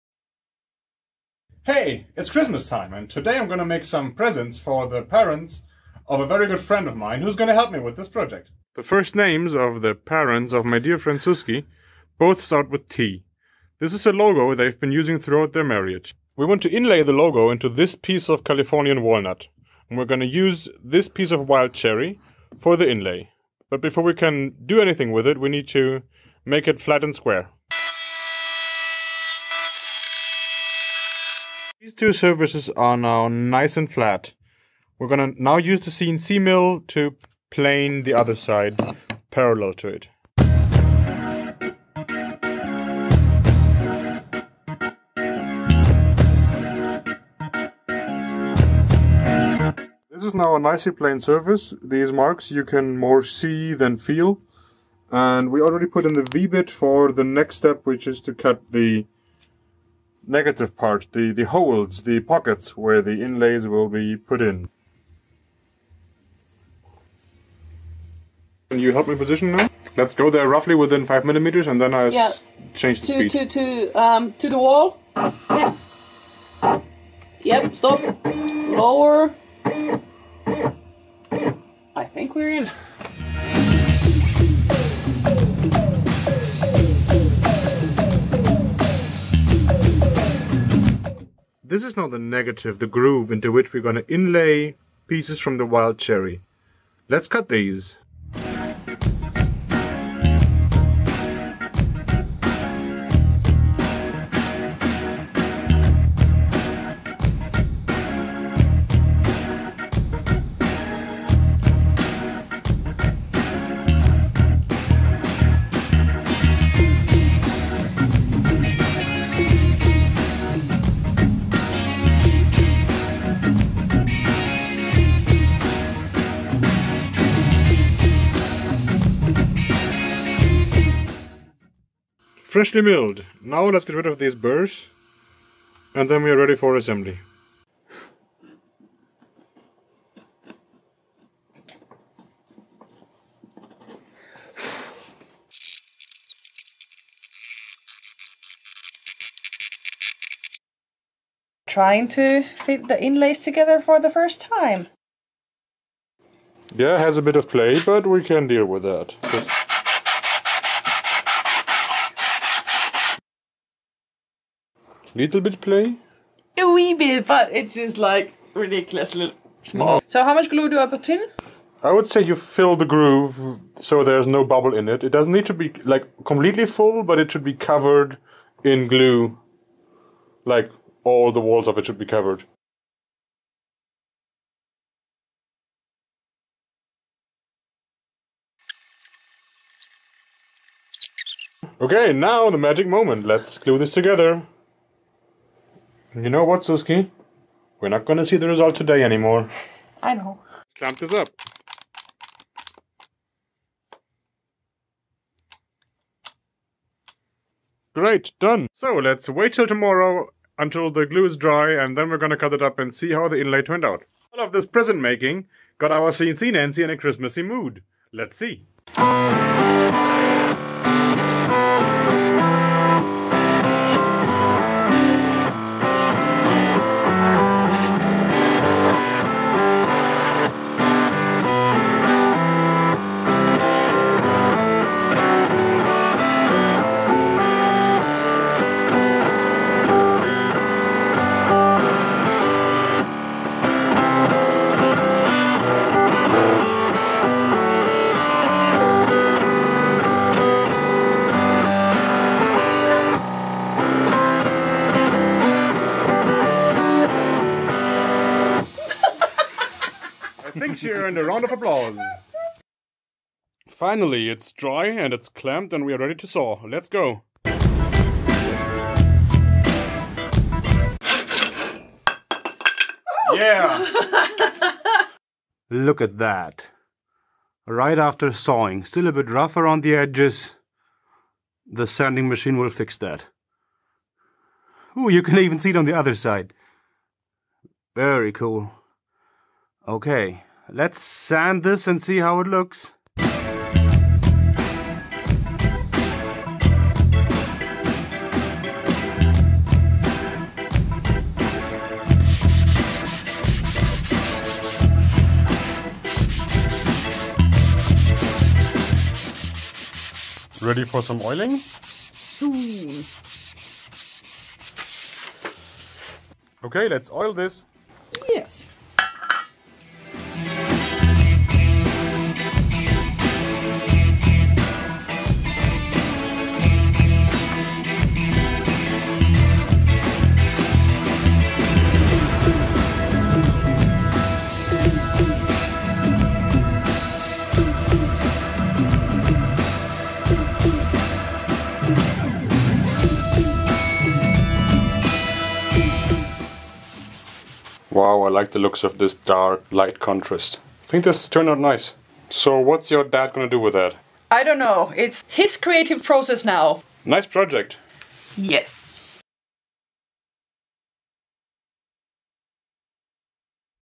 CNC GOES CHRISTMAS-CRAZY! (Suski and I make a christmas present)
Our process to create inlays with our DIY CNC mill c-nancy, using the software Inkscape and F-Engrave, also a small surprise when our cnc mill goes crazy.

The wood is walnut and wild cherry.

Thanks to Kevin McLeod for the Music.